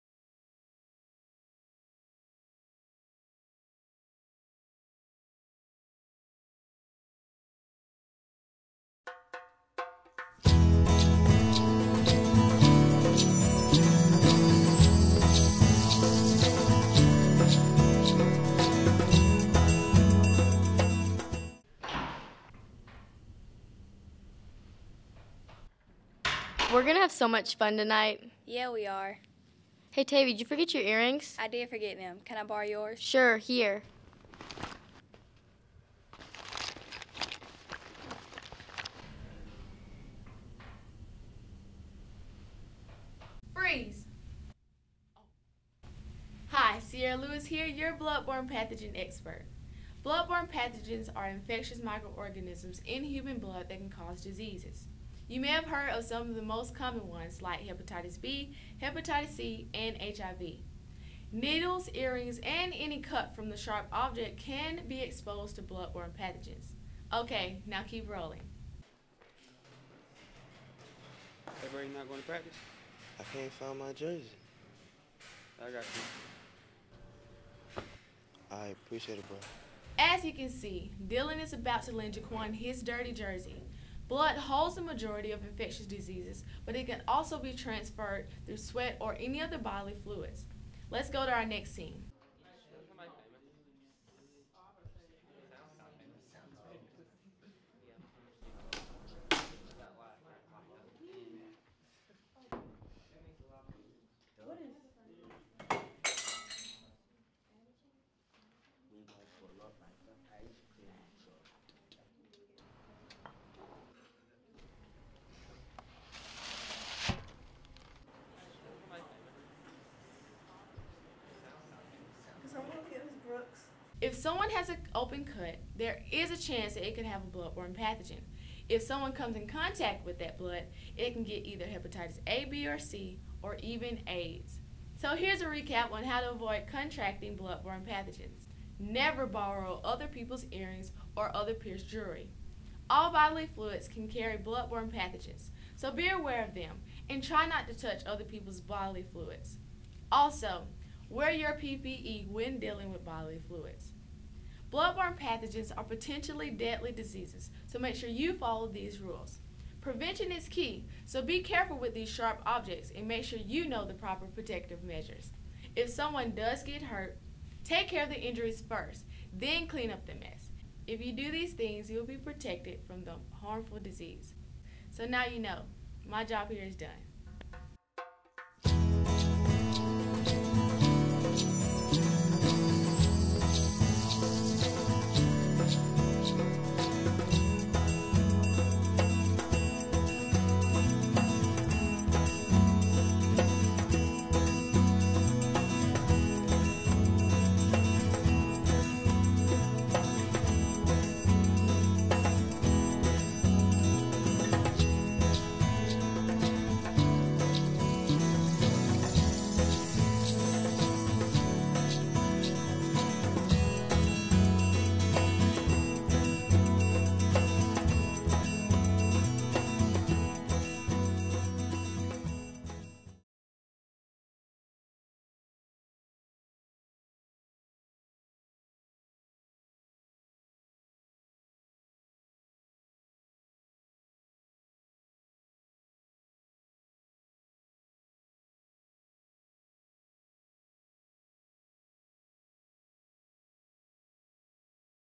Bloodborne Pathogens
Carroll High School students, in Ozark AL, produce a commercial s3D video product, on Bloodborne pathogens, using an integrated s3D graphics, and stereoscopic video production lab, designed and installed by Vizitech USA.